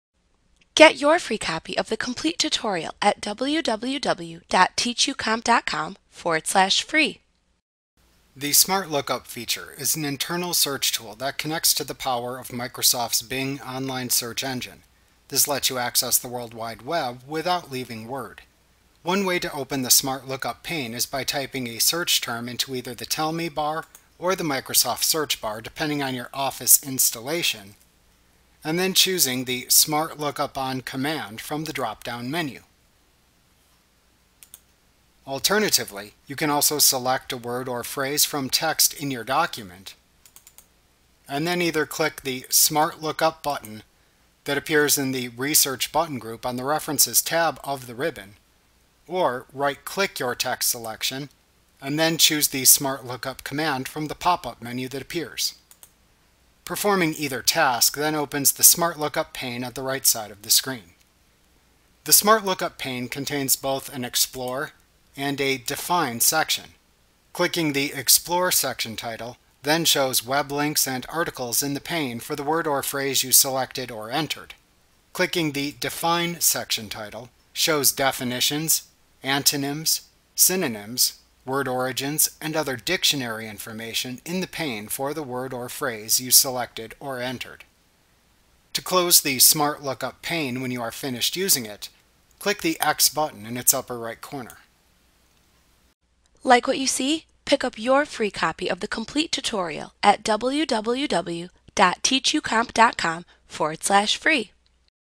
Word 2019 and 365 Tutorial Smart Lookup Microsoft Training
FREE Course! Learn about Smart Lookup in Microsoft Word 2019 & 365 - the most comprehensive Word tutorial available. Visit us today!